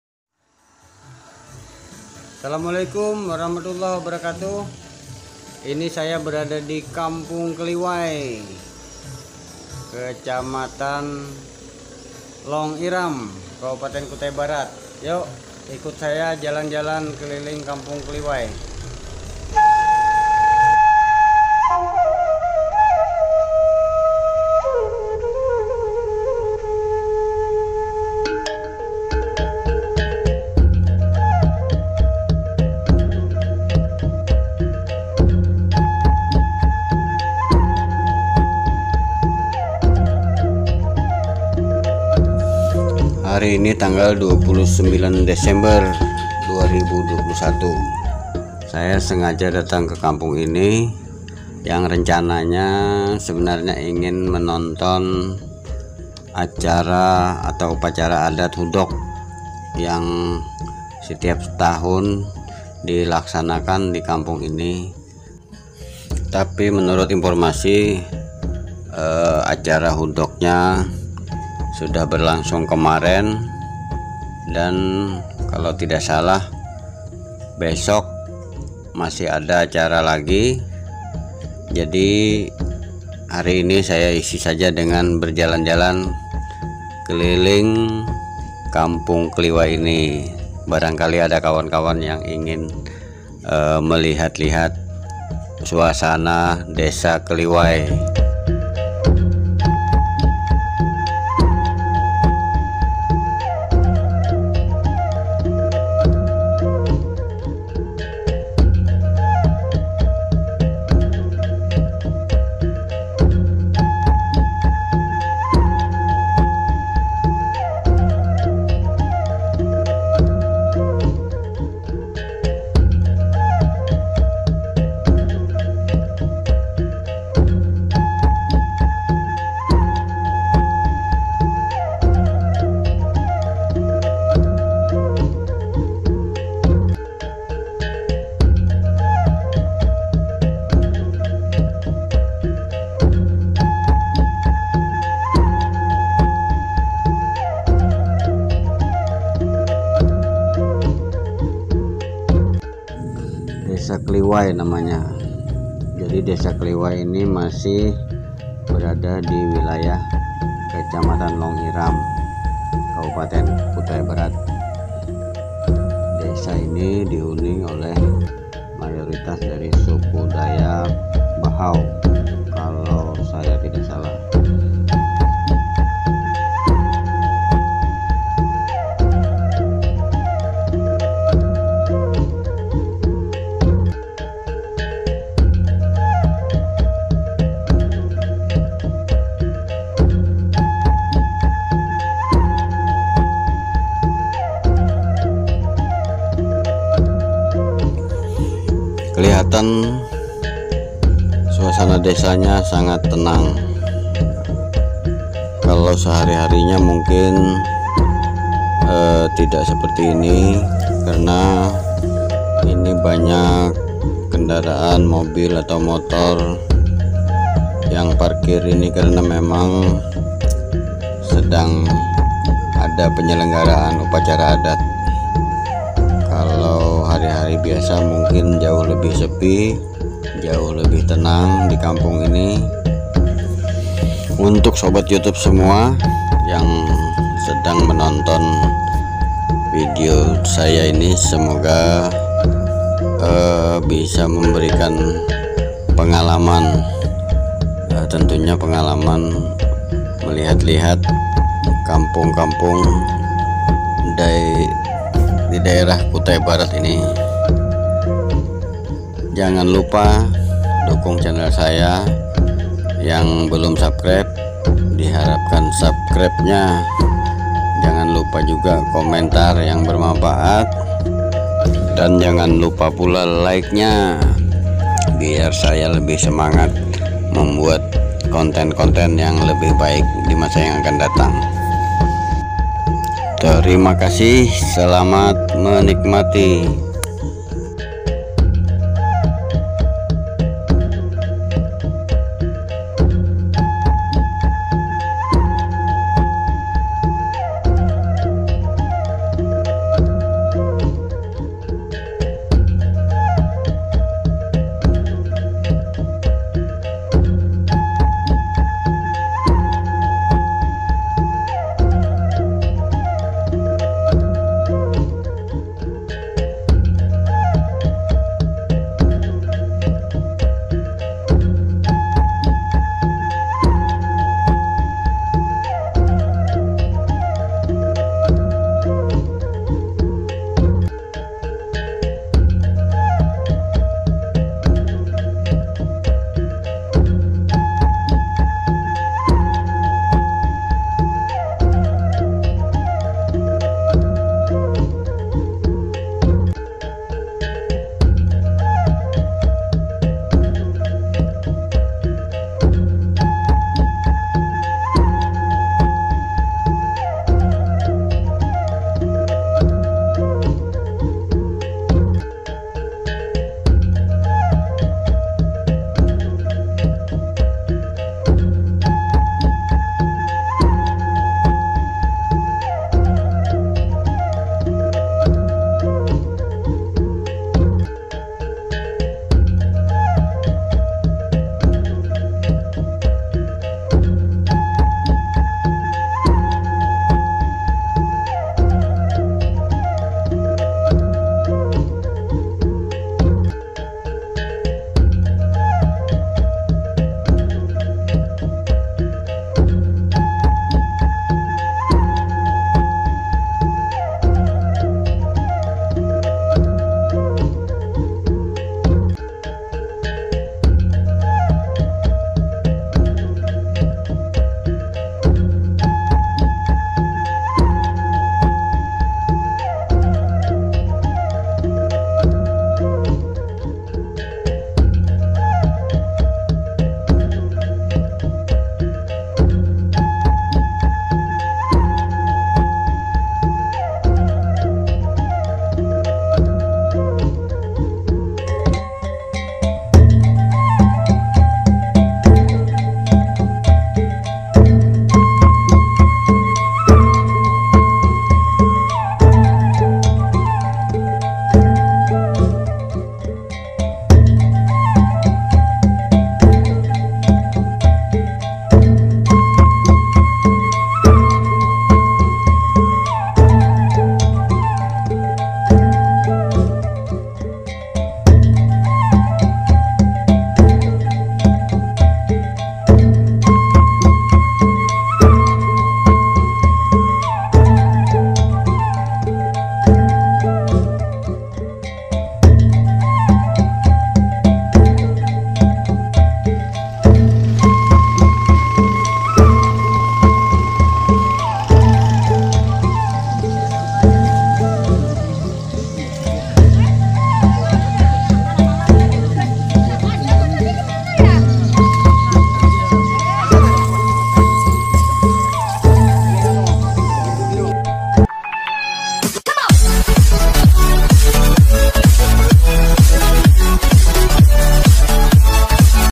Vlog keliling Kampung Keliwai || Kampung Dayak Bahau
Vlog keliling Kampung Keliwai,
 Kampung Dayak Bahau, Kampung Keliwai adalah Kampung Dayak Pedalaman sungai Mahakam, Kecamatan Long Iram, Kabupaten Kutai barat, Kalimantan timur